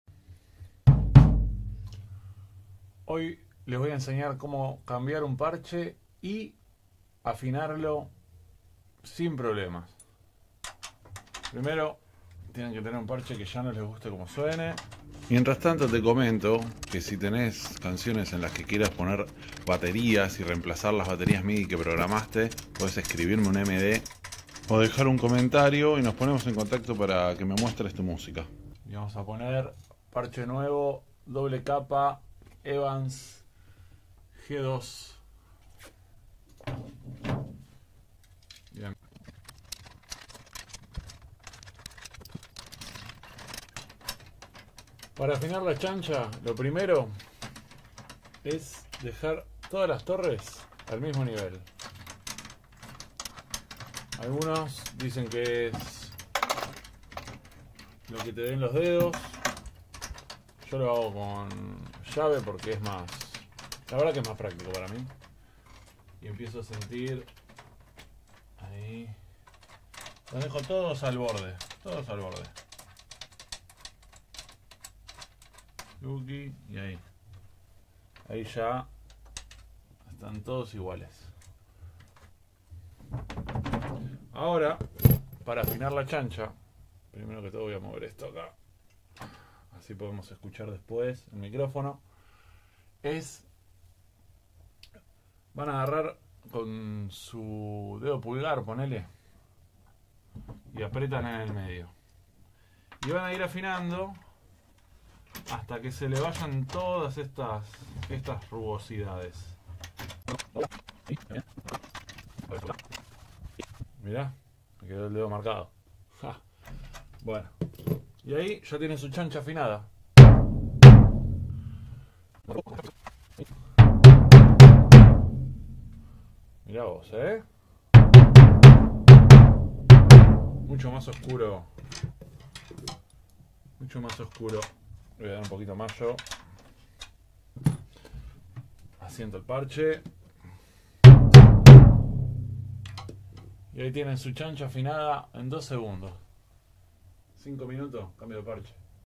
como afinar un tom de batería
Pequeña ayuda para los que  no saben como hacer que suene un parche nuevo apenas lo cambias